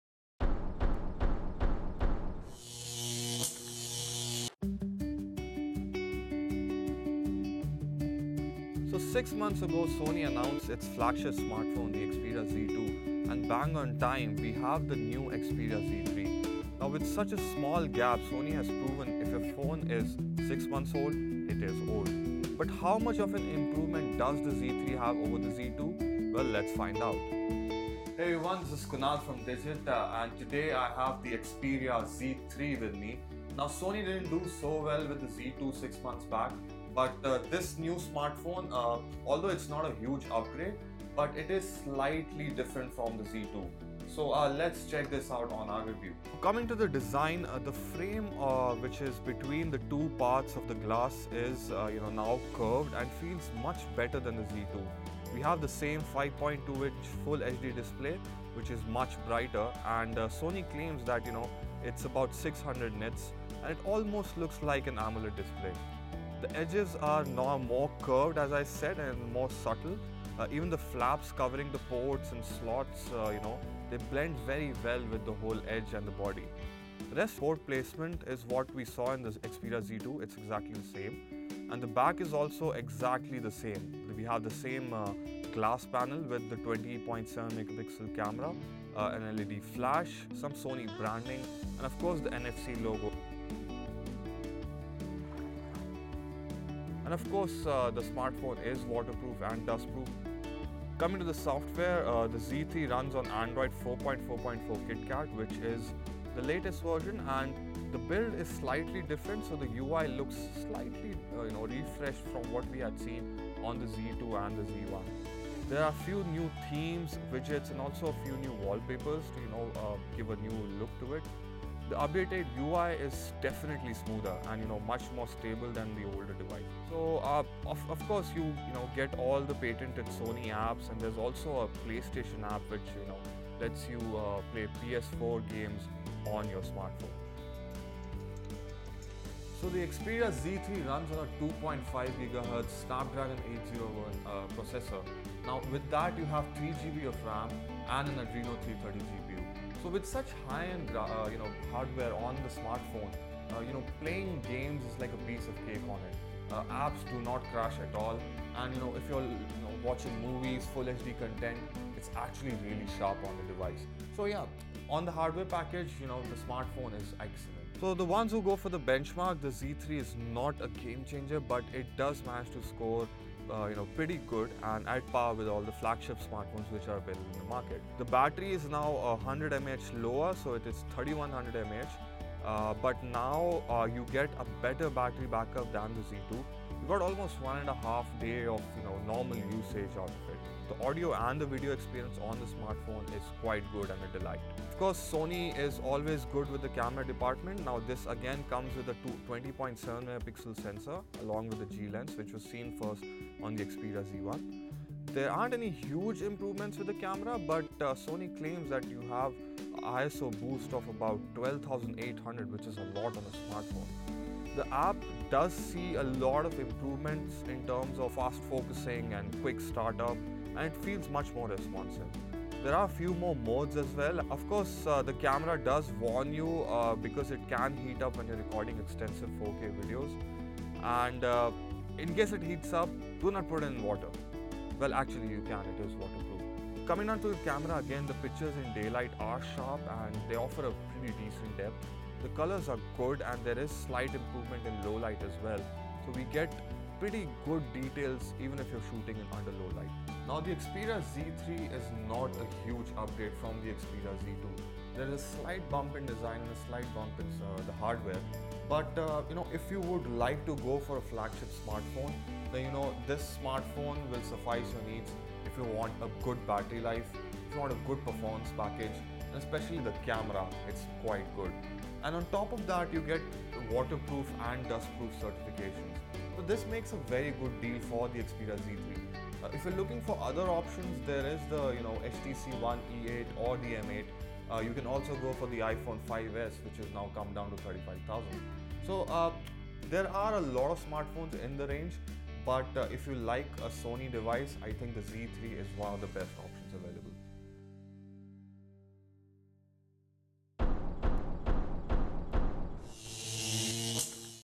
Sony Xperia Z3 Review
The Xperia Z3 maintains Sony's pedigree of great smartphones and while it does not come with any major changes when compared to the Z2, it is still a worthy flagship smartphone. It has some minor upgrades which makes it better than the Z2 and it also comes with a best in class camera and great battery life. The performance capability of the smartphone is immense and is at par with the other flagship smartphones. Would we recommend the Xperia Z3? Definitely.